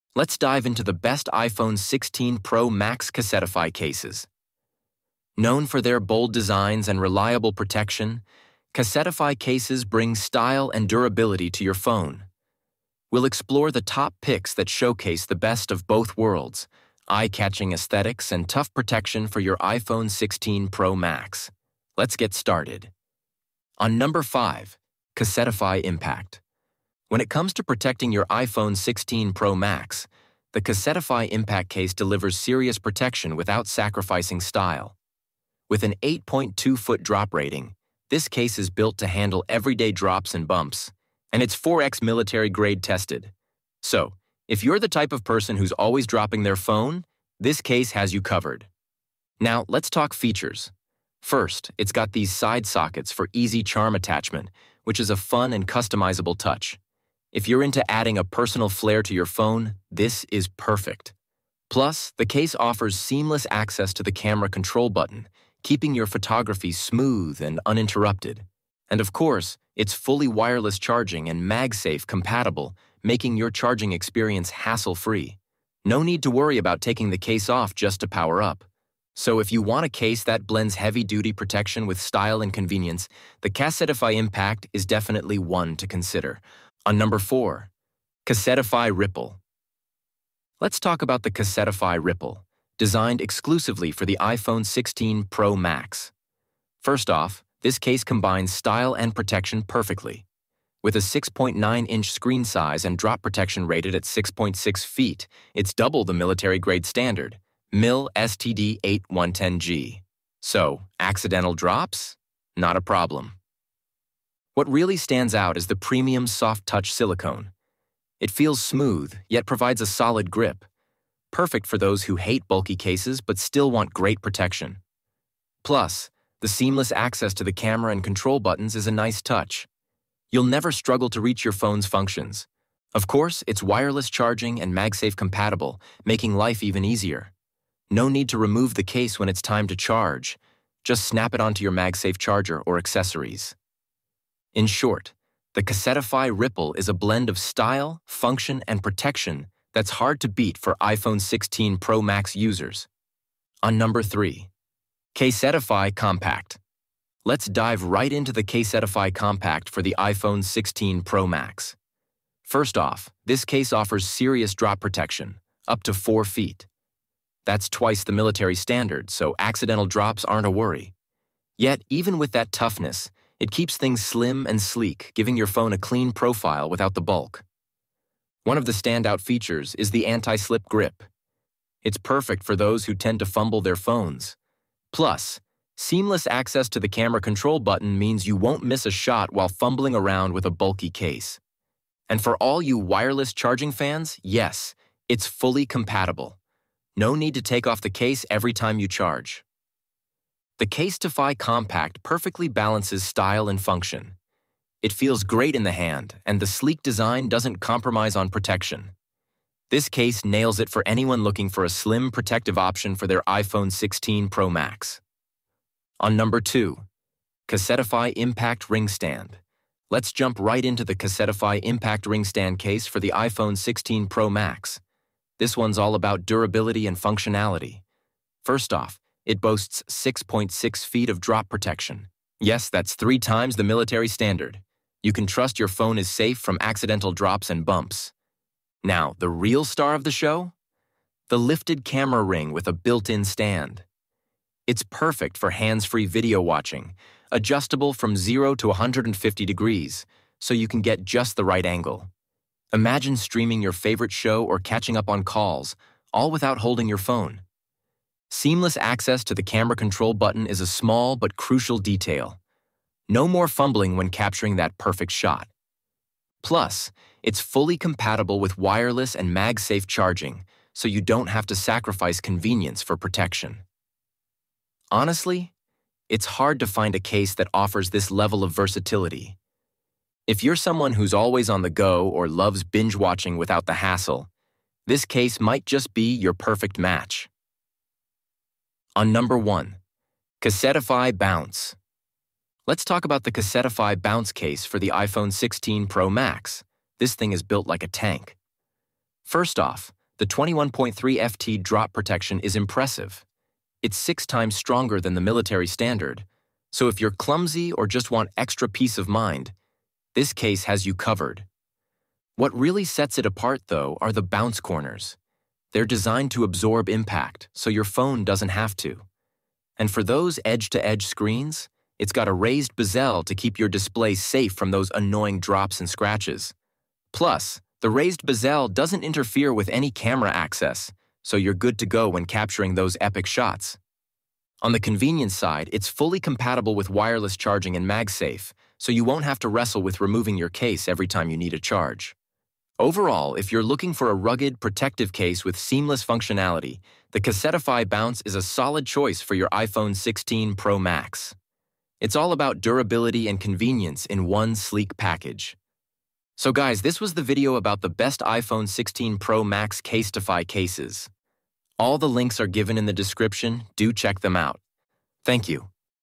Top 5 Best iPhone 16 Pro Max Casetify Cases [2025] 🔥
Let's talk about the Best iPhone 16 Pro Max Casetify Cases. I have made a list of Best iPhone 16 Pro Max Casetify Cases which will help you decide which one to choose.

Quick Links: ✅✅✅

5: CASETiFY Impact 0:23
4: CASETiFY Ripple 1:26
3: CASETiFY Compact 2:33
2: CASETiFY Impact Ring Stand 3:35
1: CASETiFY Bounce 4:48

I have prepared this video guide to Best iPhone 16 Pro Max Casetify Cases to give you the ins and outs to help you make the right choice. So let's get started.